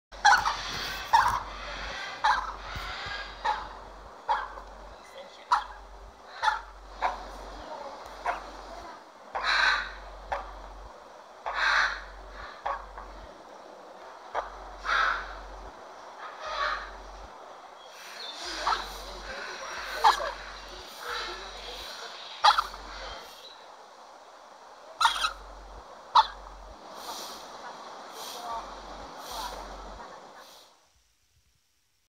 Baby Flamingo Wears Blue Boots
Baby Flamingo "Squish" is given blue boots to protect it's feet from the heat.